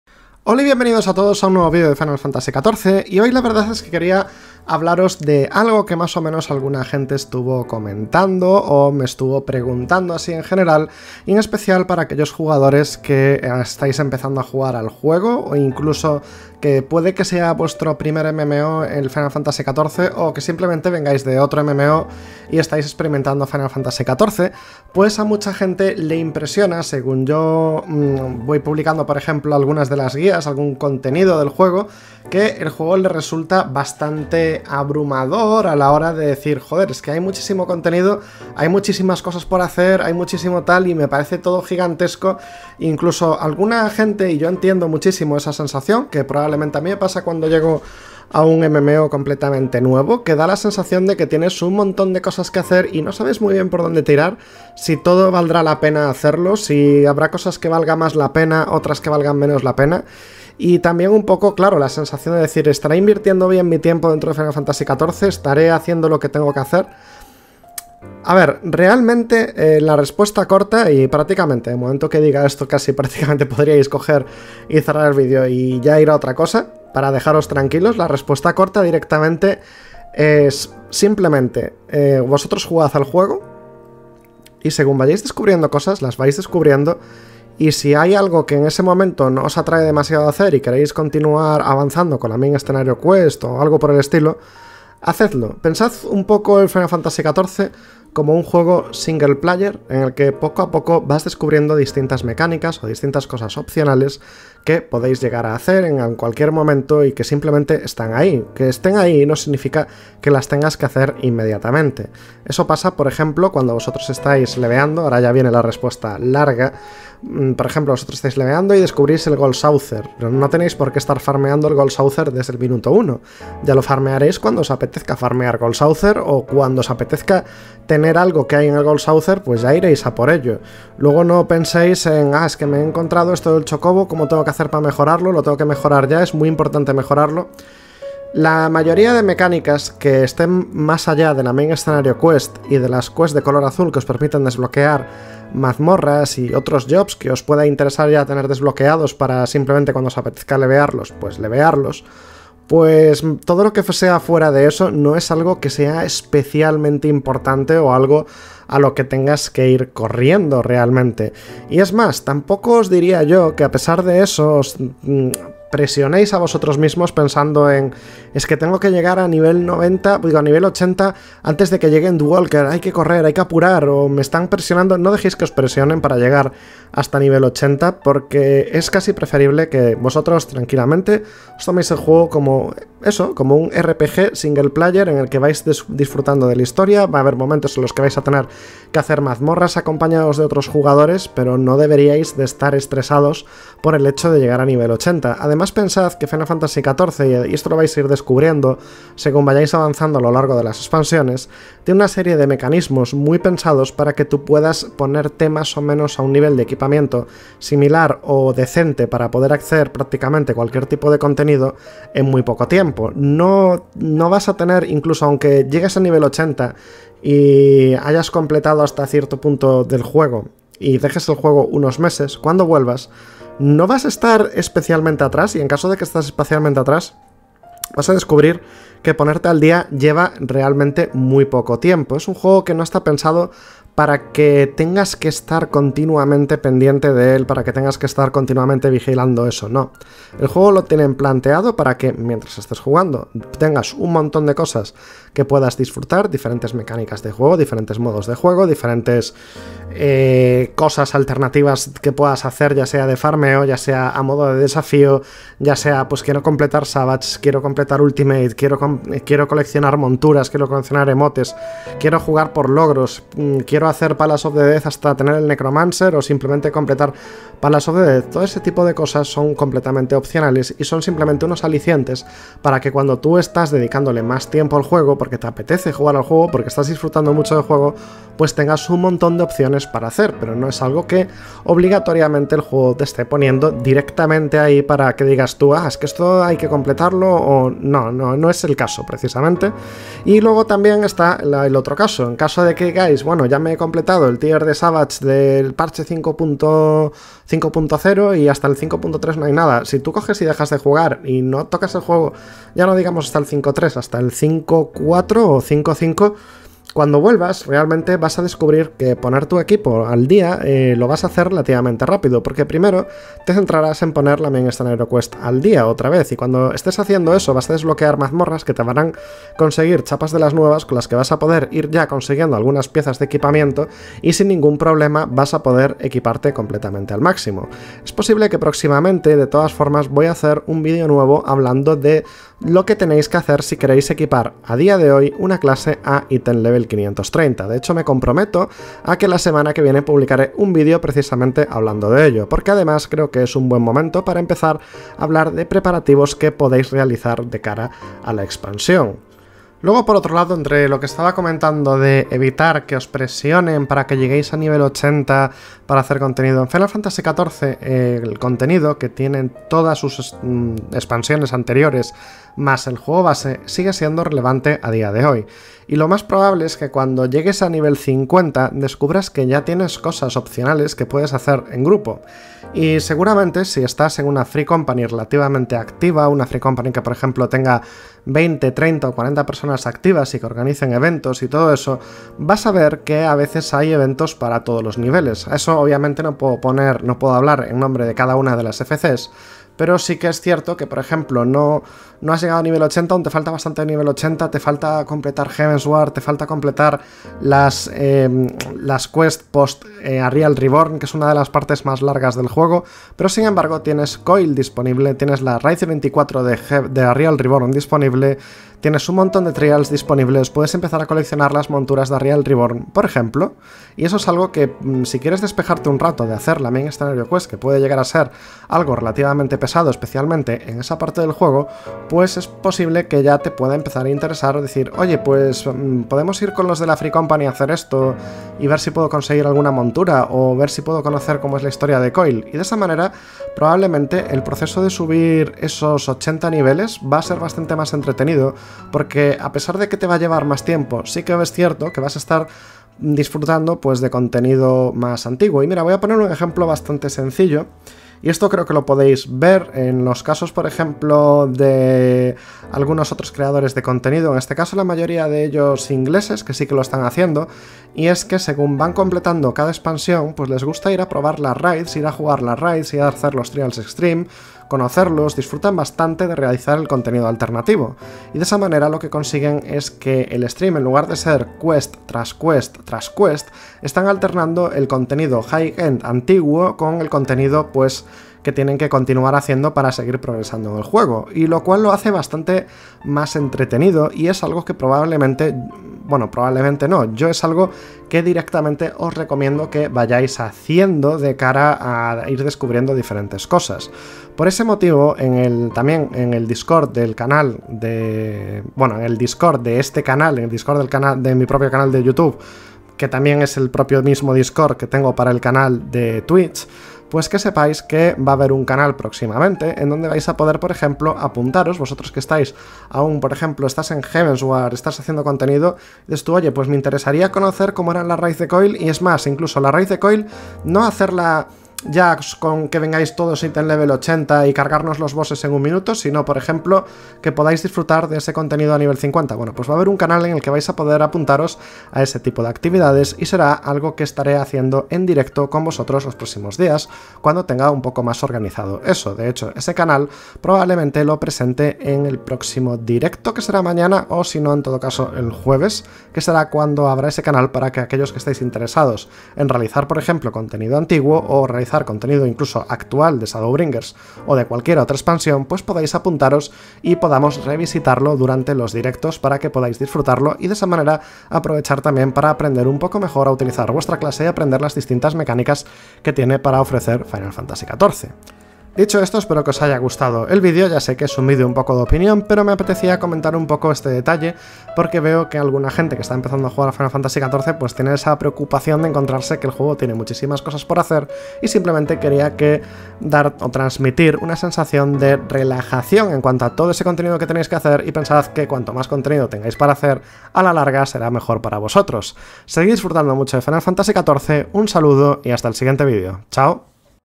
FFXIV: ¿Te abruma la cantidad de cosas por hacer en FFXIV? Mira éste vídeo ;)
No llevas mucho tiempo jugando a Final Fantasy XIV y te asusta la enorme cantidad de contenido que tiene Final Fantasy XIV?. En éste vídeo te cuento mi opinión acerca de ello y algunos consejos para que ese volumen de contenido en ningún momento de abrume, para que veas el juego desde otro punto de vista y veas que no todo necesita hacerse inmediatamente ni todo requiere de tu tiempo. Es un vídeo de pura opinión personal y de consejos.

🐦 Para estar atentos a las novedades del canal y otras chorradas que diga siempre podeis seguirme en Twitter

Final Fantasy XIV es un MMORPG desarrollado por Square-Enix dirigido y producido actualmente por Naoki Yoshida y que está disponible tanto para Playstation 4, Playstation 5, como para PC y Mac.